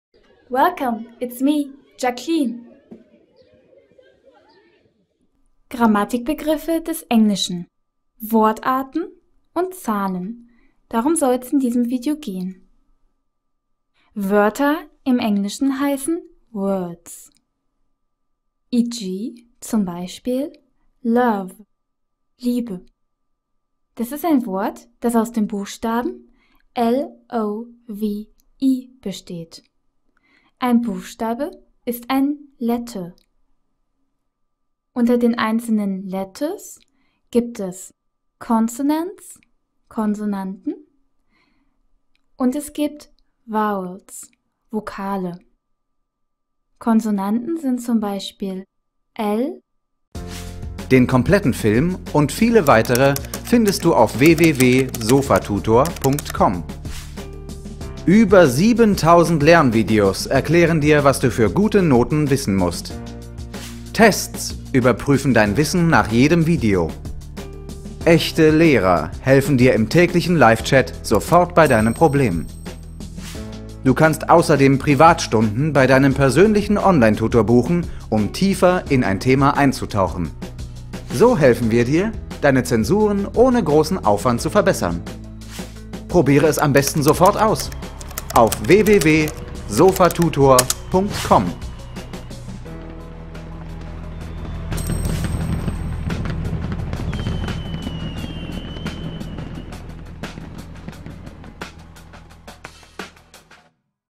In diesem Video geht es um die Namen englischer Grammatikbegriffe und der Zahlen | Englisch
Hallo und herzlich willkommen zu einem neuen Video, dass dir beim Englischlernen helfen wird! Im Englischunterricht fallen sicherlich immer wieder eine ganze Reihe von grammatikalischen Fachausdrücken, die gar nicht so einfach zu verstehen sind. Dieses Video hilft dir dabei, solche Grammatikbegriffe des Englischen zu lernen. Genauer gesagt, geht es heute um die Wortarten und die Zahlen. Begriffe wie auxiliary (Hilfsverb), modal (Modalverb) oder preposition (Präposition) werden schon bald kein Problem mehr für dich sein! Viel Spaß dabei!

Dieses Video zum Thema Wortarten gehört zu: Englisch | Grammatik | Wortarten